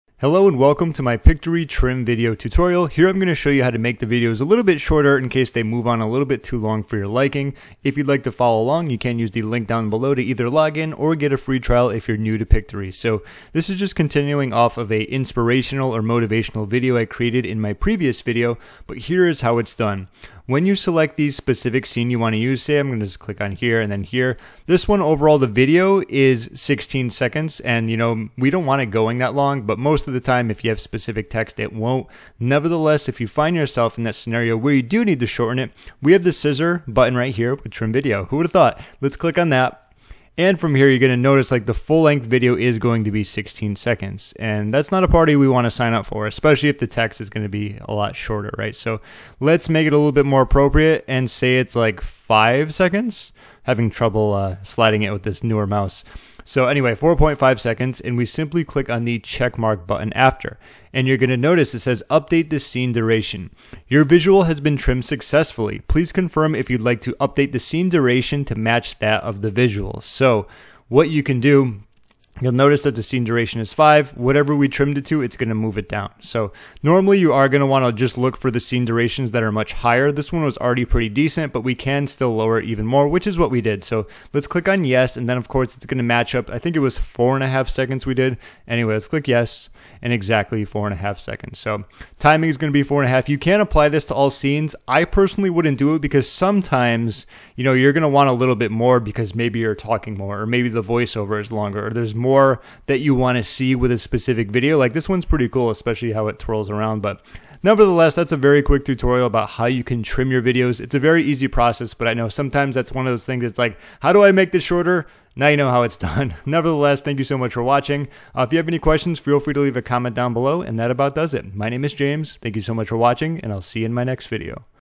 Pictory Trim Video (How To Edit Your Scene Duration & Trim Video)
Here is how to create a script for YouTube videos, so that you can plug it in and use it with Pictory. I'll be giving you a prompt/template that is very easy to fill in. As long as you know what you want out of your video, you should have no problem getting a decent script.